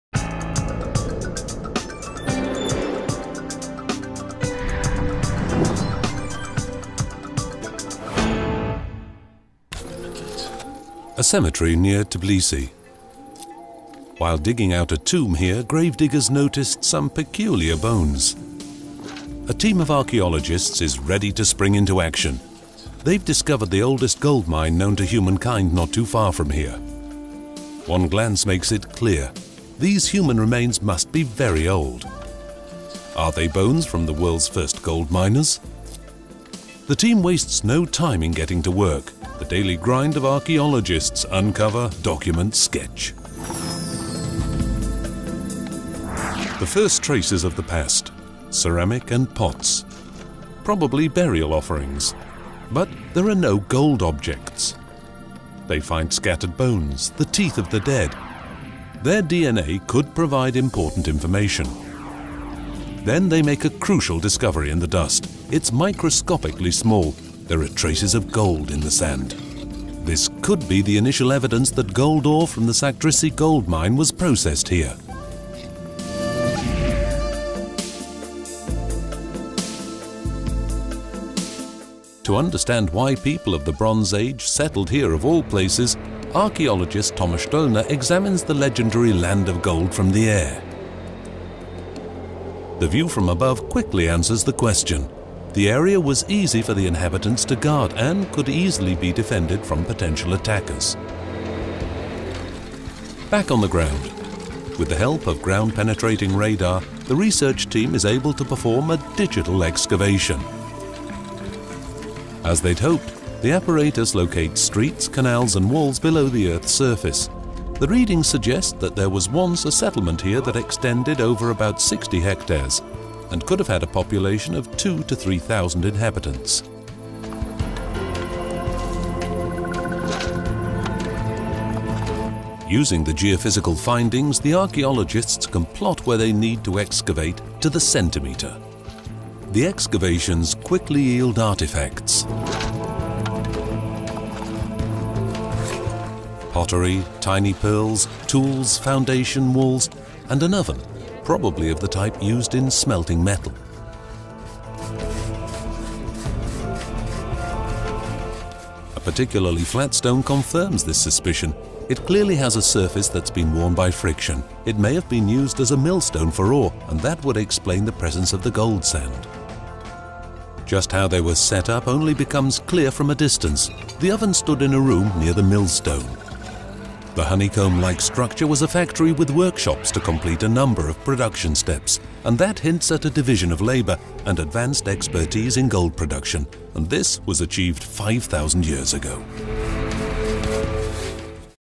View archaeologists uncover traces of Bronze Age gold workshops in a cemetery near Tbilisi, Georgia
Watch archaeologists uncover traces of a Bronze Age metallurgical workshop in Georgia.
The first traces of the past: ceramic and pots - probably burial offerings. But there are no gold objects. They find scattered bones, the teeth of the dead. Their DNA could provide important information. Then they make a crucial discovery in the dust. It is microscopically small. There are traces of gold in the sand. This could be the initial evidence that gold ore from the Sakdrissi gold mine was processed here.

To understand why people of the Bronze Age settled here, of all places, archaeologist Thomas Stöllner examines the legendary land of gold from the air. The view from above quickly answers the question. The area was easy for the inhabitants to guard and could be easily defended from potential attackers. Back on the ground, with the help of a ground-penetrating radar the research team is able to perform a digital excavation. As they'd hoped, the apparatus locates streets, canals and walls below the earth's surface. The readings suggest that there was once a settlement here that extended over about 60 hectares and could have had a population of 2,000-3,000 inhabitants.